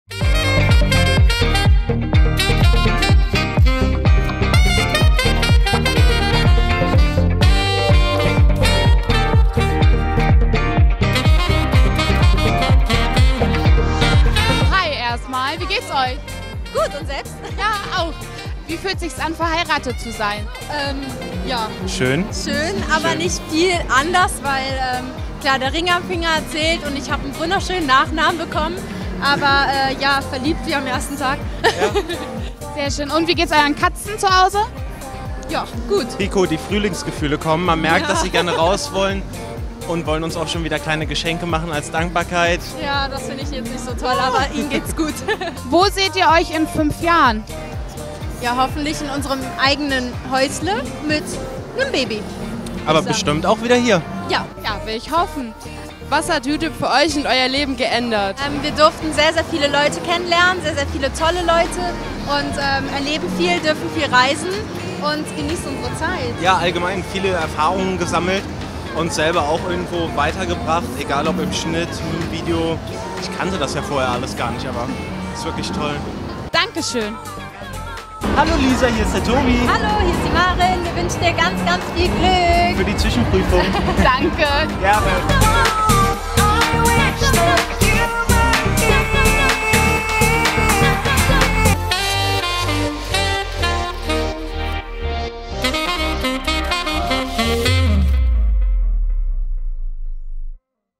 GLOW The Beauty Convention by dm - Maren und Tobi Wolf im Interview
Während der GLOW - The Beauty Convention by dm in Dortmund haben die Youngsters die Gelegenheit genutzt, einige Youtuber und Influencer zu interviewen.

Kim und Hannah hatten auf der GLOW in Dortmund die Gelegenheit, Maren und Tobi Wolf zu befragen. Es geht um das Paar, ein Haus und ein Baby ;-)